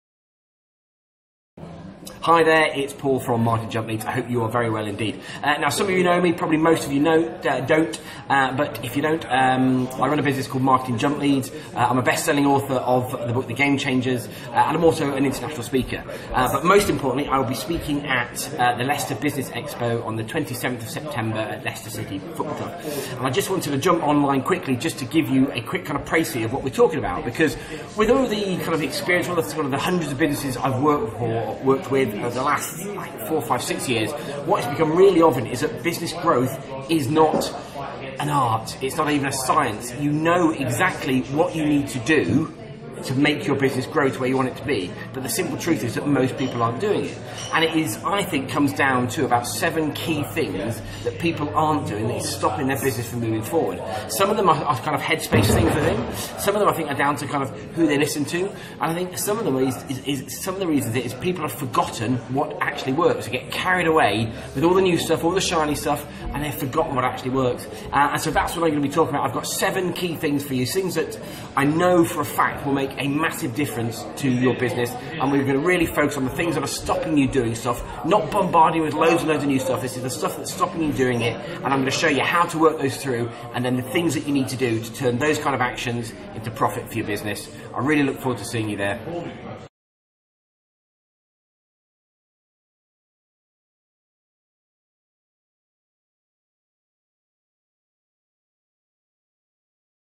Paul Chapman at LBE 2018
Paul Chapman, speaker, author and entrepreneur will be speaking at Leicester Business Expo 2018.  Here Paul talks about the key things he'll be sharing on the day.

Leicester Business Expo is on 27th September 2018 at King Power Stadium in Leicester